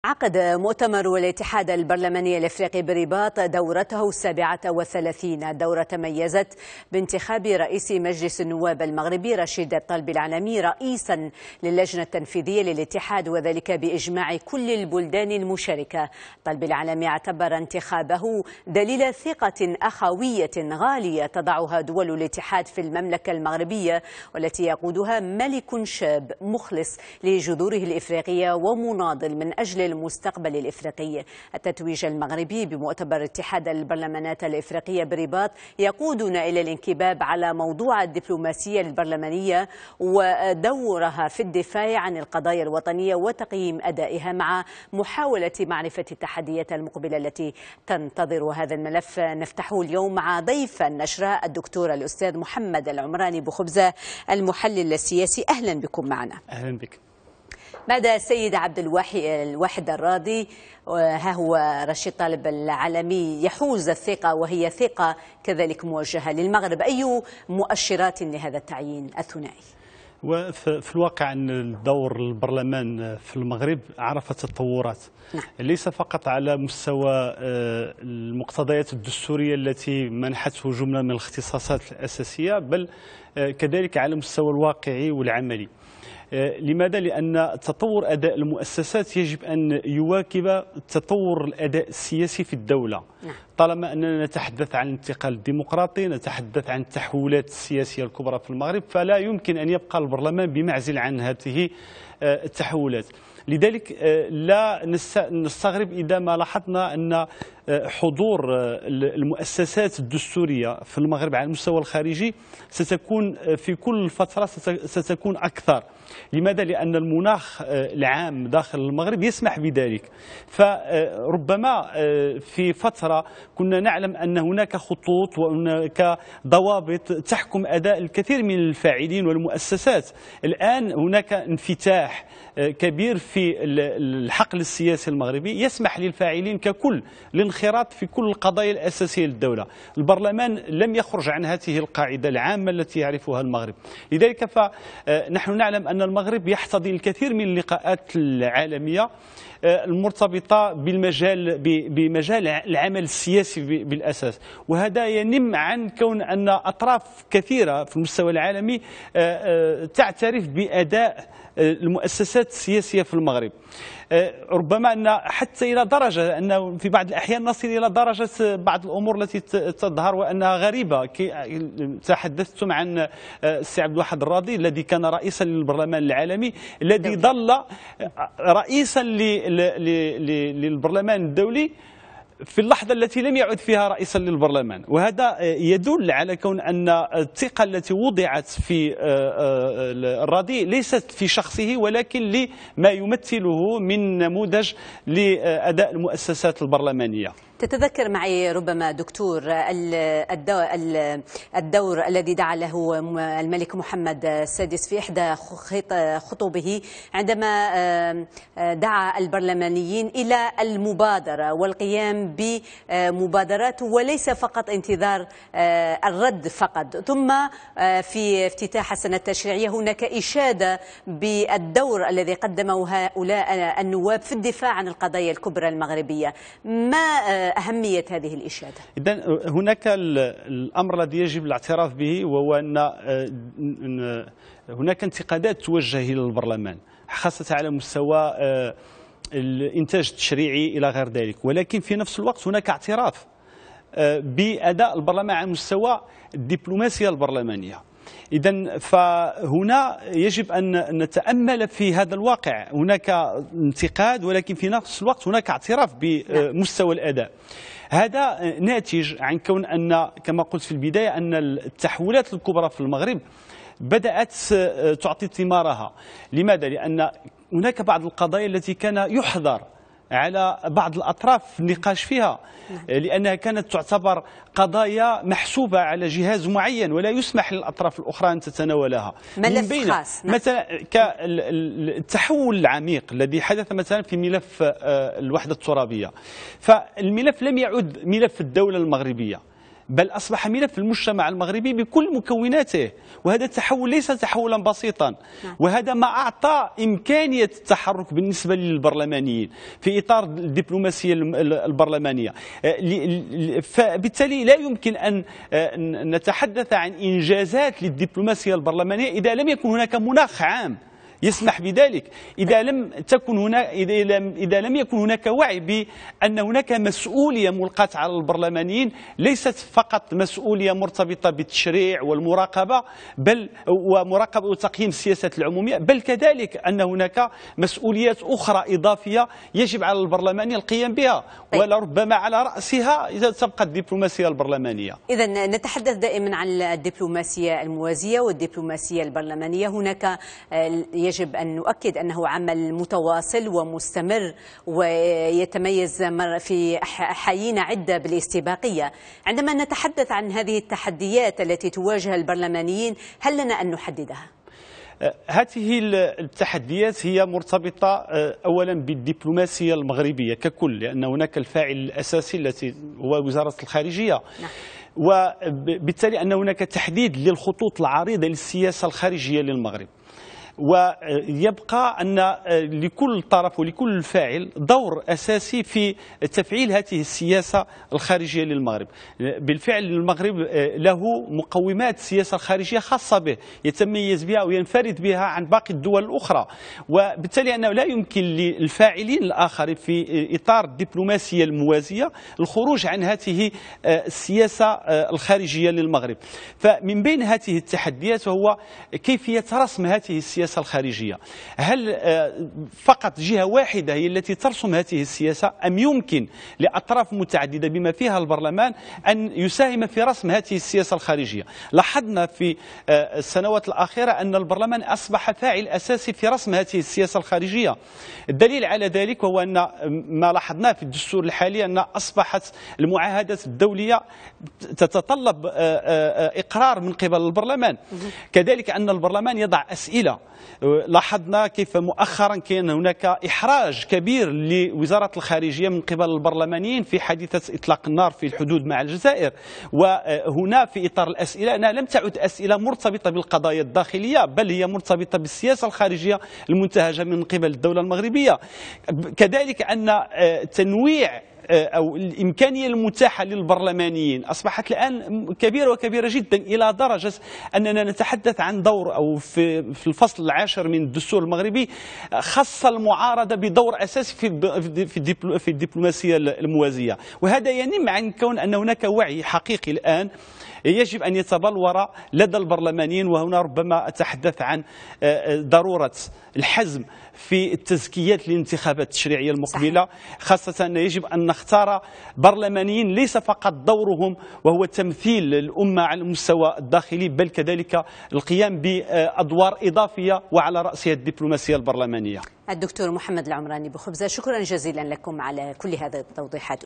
الدبلوماسية البرلمانية والدفاع عن القضايا الوطنية
عقد مؤتمر الإتحاد البرلماني الإفريقي بالرباط دورتَه السابعة و الثلاثين. الدورة تميزت بانتخاب رئيس ِمجلس النواب المغربي رشيد الطالبي العلمي رئيسا ً للجنة التنفيذية للإتحاد وذلك بإجماع ِ كل البلدان المشاركة.
الطالبي العلمي اعتبر انتخابَه دليل َ ثقة أخوية غالية تضعها دولُ الإتحاد في المملكة المغربية و التي يقودها ملك شاب مُخلِص لجذوره الإفريقية ومناضل من اجل المستقبل الإفريقي.
التتويج المغربي بمؤتمر الإتحاد البرلماني الإفريقي بالرباط يقودنا إلى الإنكباب على موضوع الدبلوماسية البرلمانية، دورها في الدفاع ِعن القضايا الوطنية وتقييمُ أدائها، مع محاولةِ  معرفة التحديات المقبلة التي تنتظرها.
هذا الملف نفتحه مع ضيفنا محمد العمراني بوخبزة المحلل السياسي.